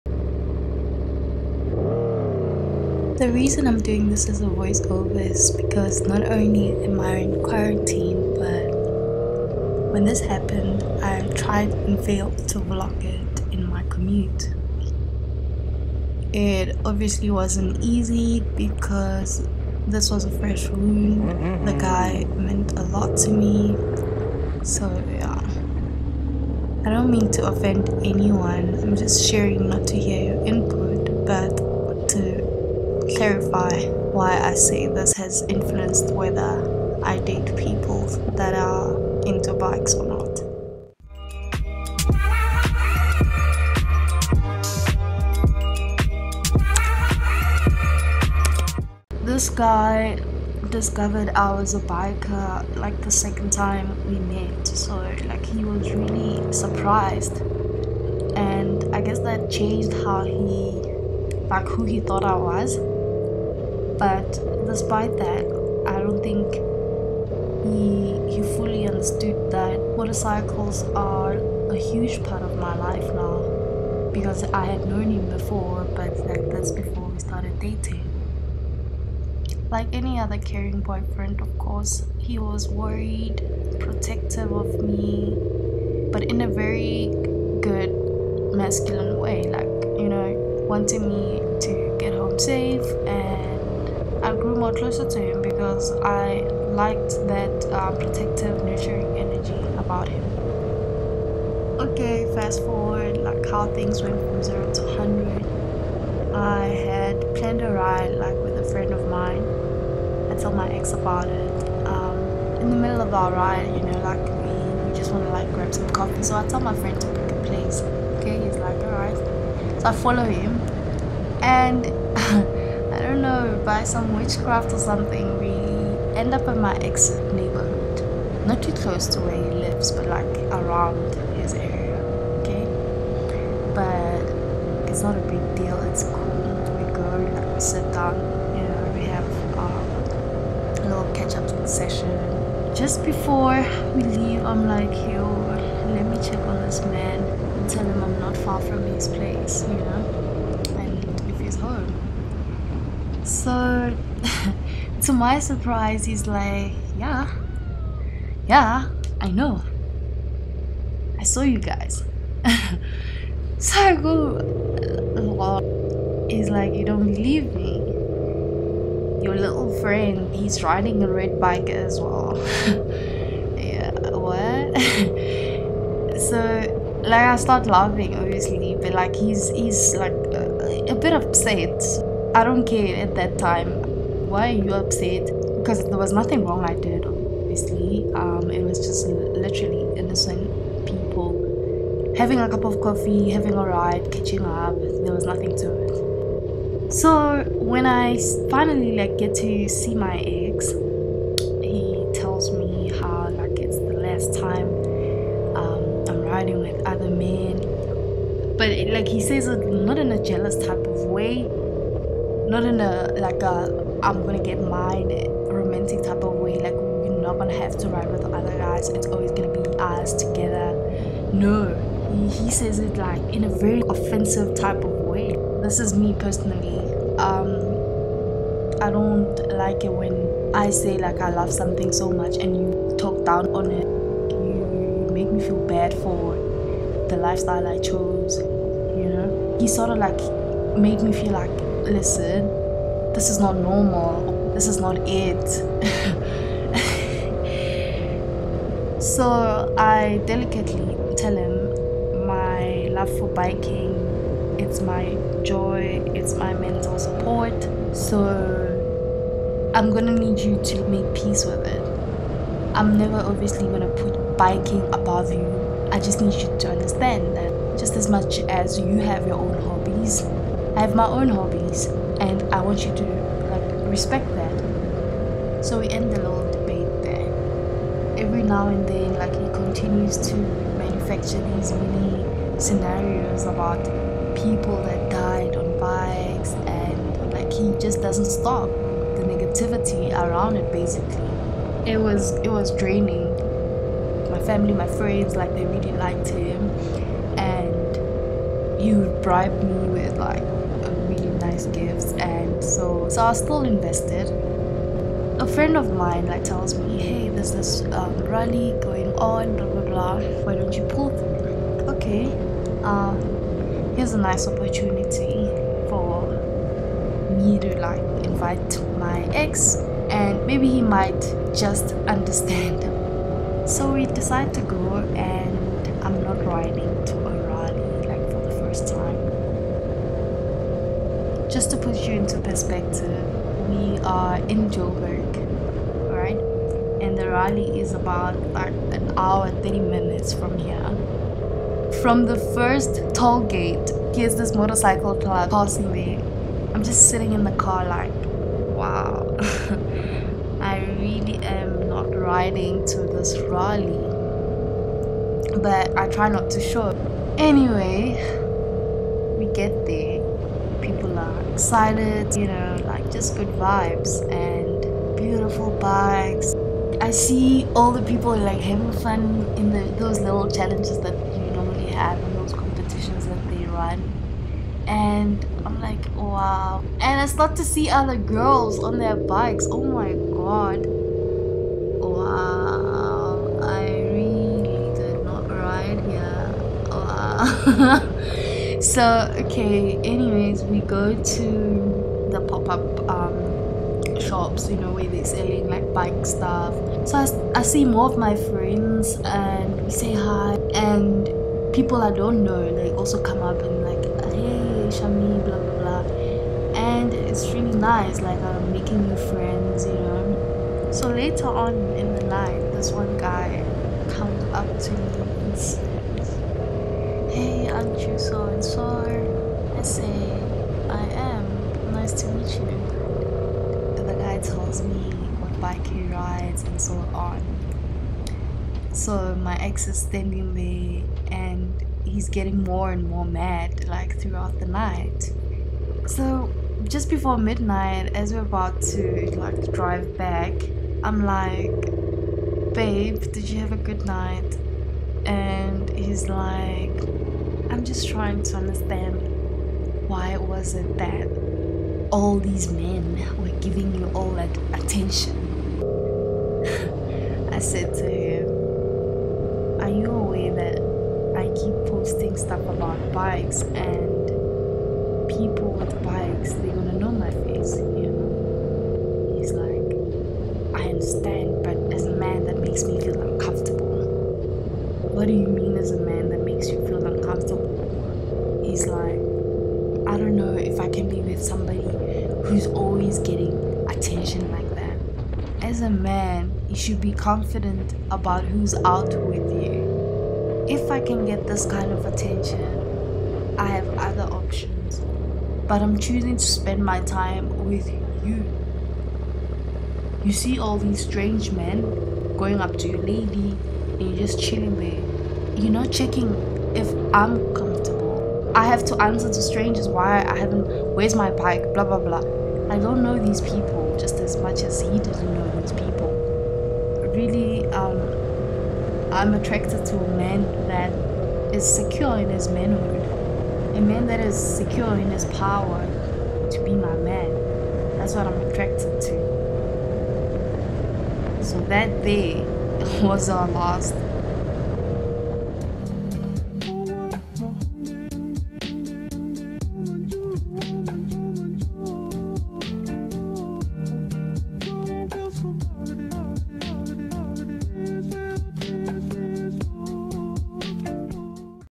STORY TIME : Biker Girl Dating Struggles | South African YouTuber
Those with no experience of what we do find it very easy to be scornful.
But those of us who have loved and lived a bike life for many years of devotion are aware of so much more.

I appreciate all your support.God bless!

𝐍𝐞𝐱𝐭 𝐮𝐩𝐥𝐨𝐚𝐝 :
Tomorrow 21H00 CAT.